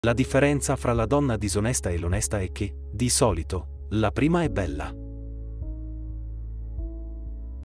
Citazioni di Roberto Gervaso
Le Migliori Citazioni di Roberto Gervaso

Immergiti nell'incantevole mondo delle parole con 'Le Migliori Citazioni di Roberto Gervaso'! Questo emozionante video su YouTube ti porterà in un viaggio poetico, in cui la saggezza e l'intensità delle parole di Gervaso si fonderanno con la bellezza visiva. Avvolgiti nella dolce melodia delle sue citazioni, che risveglieranno i sentimenti più profondi e ti condurranno in un universo di emozioni autentiche. Lasciati trasportare dalla magia delle parole, mentre Gervaso dipinge quadri di significato con la sua eloquenza senza pari. Preparati a essere affascinato, ispirato e toccato nel profondo da questo straordinario viaggio attraverso la mente e il cuore di uno dei più grandi autori contemporanei. Scopri 'Le Migliori Citazioni di Roberto Gervaso' e lascia che le sue parole diventino la colonna sonora della tua anima.